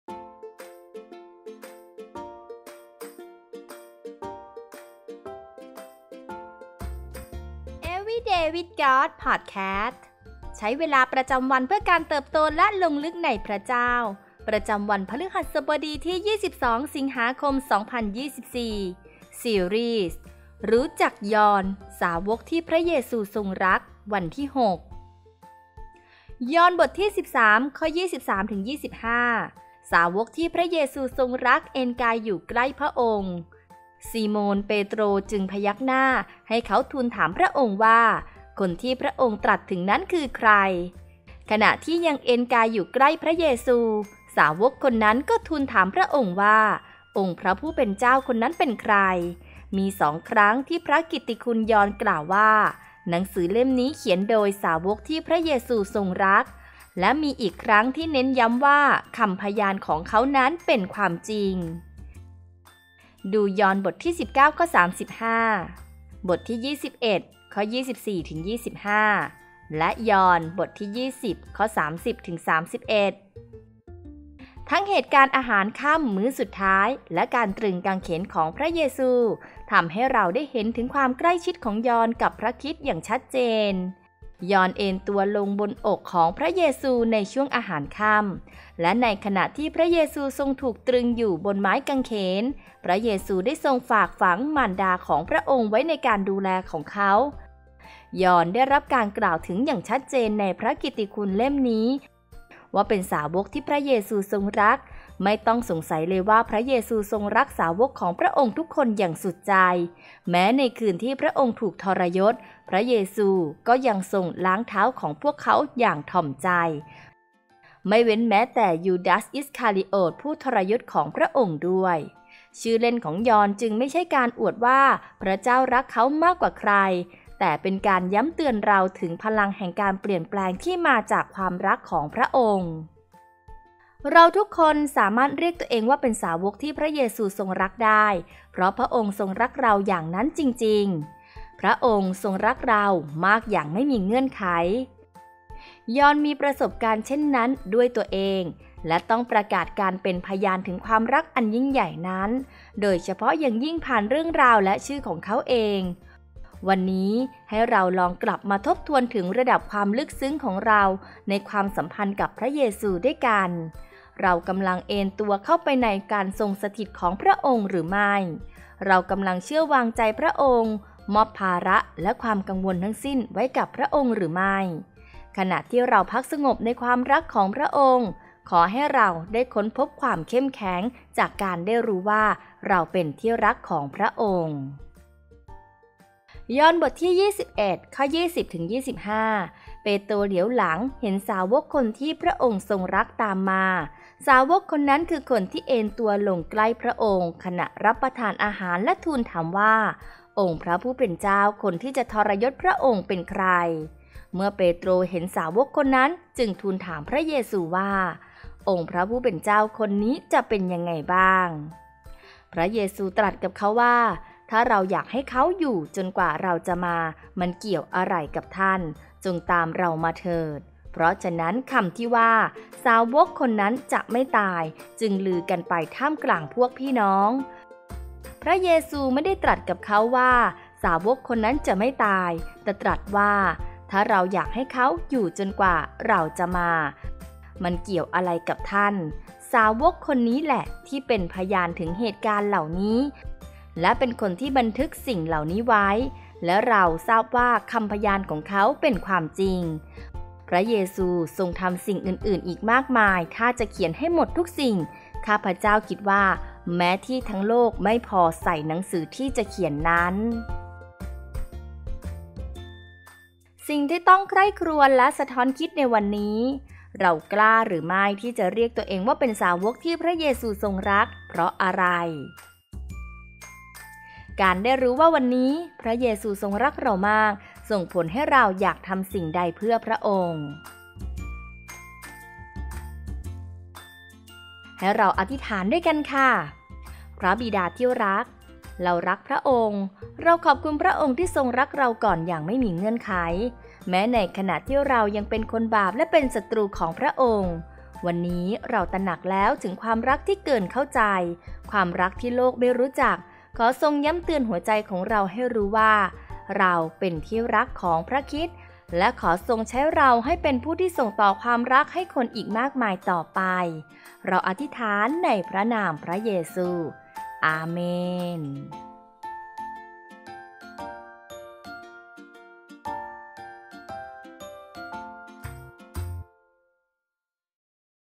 22/8/2024 Every Day with God | ซีรีส์: รู้จักยอห์นสาวกที่พระเยซูทรงรัก Introducing John | วันที่ 6/7
ประจำวันพฤหัสบดีที่ 22 สิงหาคม 2024

ซีรีส์ : รู้จักยอห์น สาวกที่พระเยซูทรงรัก
วันที่ 6 ยอห์น สาวกที่พระเยซูทรงรัก

OVERVIEW :
อัครสาวกยอห์น ซึ่งมักเรียกกันว่า ‘สาวกที่พระเยซูทรงรัก’ ถือเป็นบุคคลที่มีเอกลักษณ์โดดเด่นและเป็นที่เคารพนับถือในประวัติศาสตร์คริสเตียน  ยอห์นเกิดที่หมู่บ้านชาวประมงชื่อเบธไซดา  การเดินทางของยอห์นกับพระเยซูเริ่มต้นด้วยการทรงเรียกง่ายๆ ที่ริมทะเลสาบกาลิลี  ความเต็มใจของเขาที่จะละทิ้งอวนและเดินติดตามพระคริสต์นี่เองที่เป็นจุดเริ่มต้นความสัมพันธ์อันแสนลึกซึ้งกับพระเจ้า

บทใช้เวลากับพระเจ้านี้แปลและเรียบเรียงจากแผนการอ่าน Introducing John โดย Heidi Wysman และ Hope Media